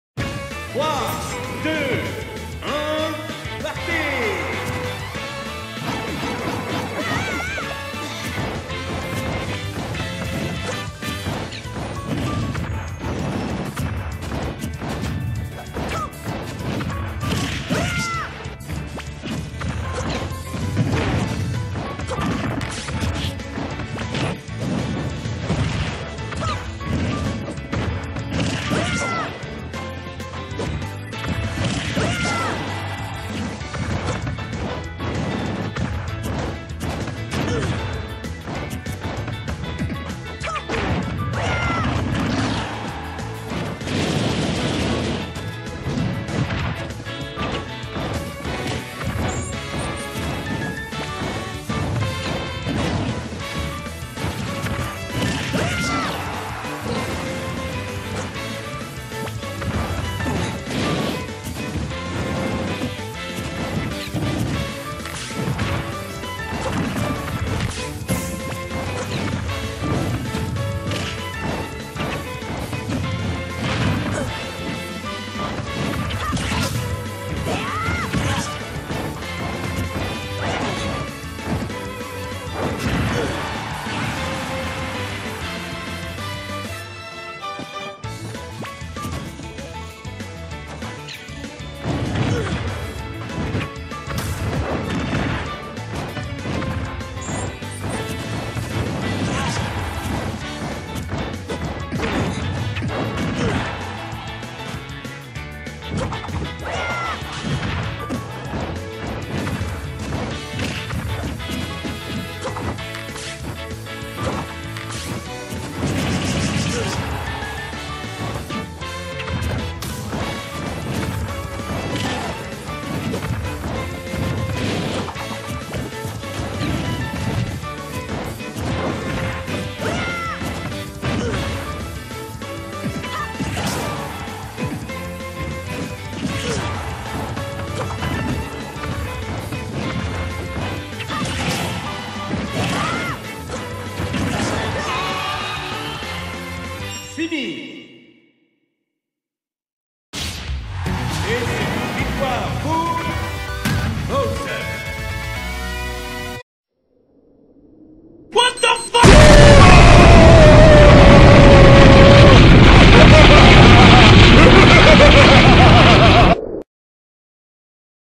Cette défaite est wtf... - Link cartoon (J1) vs Bowser (J2) duel en ligne pour la gloire
Lol on est mort en même temps mais c'est lui qui a gagné 😂